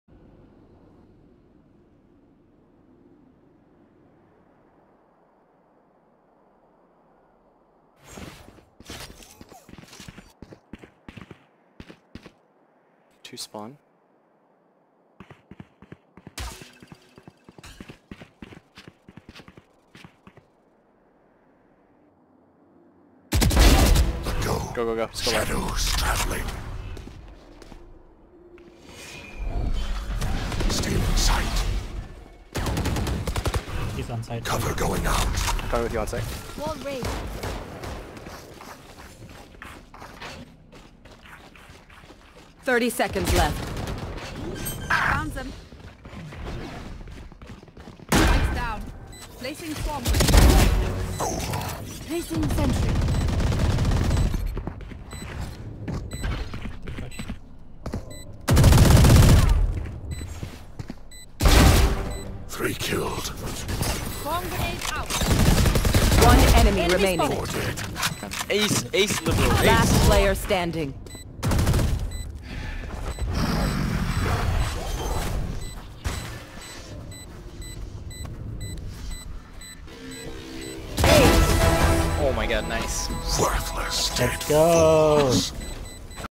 Lurk Push into Defensive Ace
Typical lurk solo push leads to a daring spike defense.